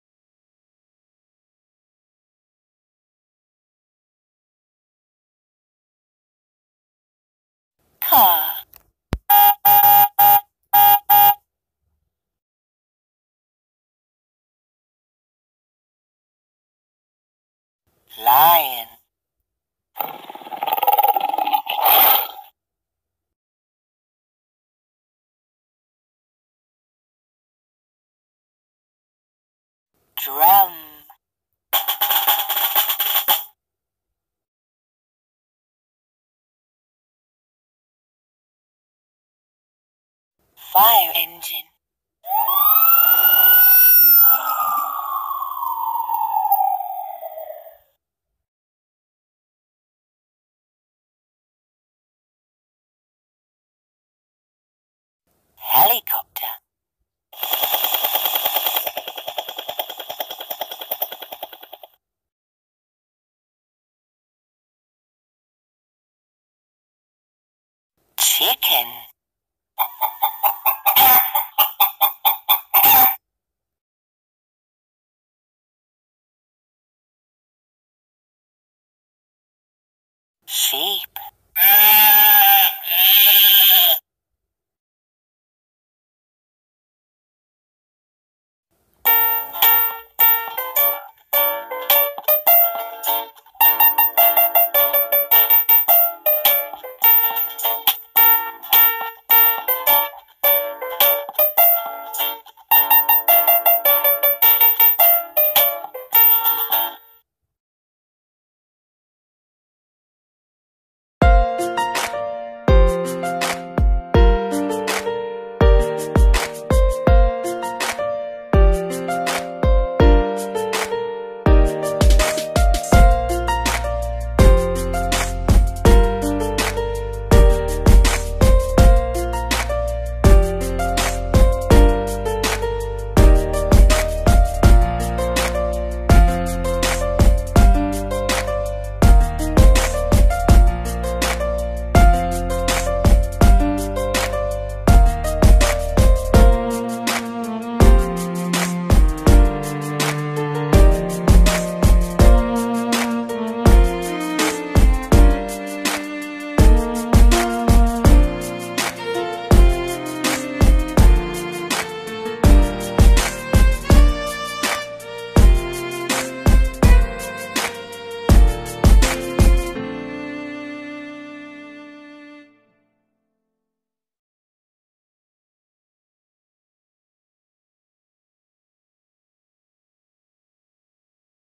First Steps Write & Wipe First Words with sounds
First Words with Sounds – First Steps Write & Wipe – Write, wipe, and learn with this educational First Steps title. Each activity helps your child learn how to identify and write common words, while the 27 fun sound buttons help them say the word and associate it with the sound.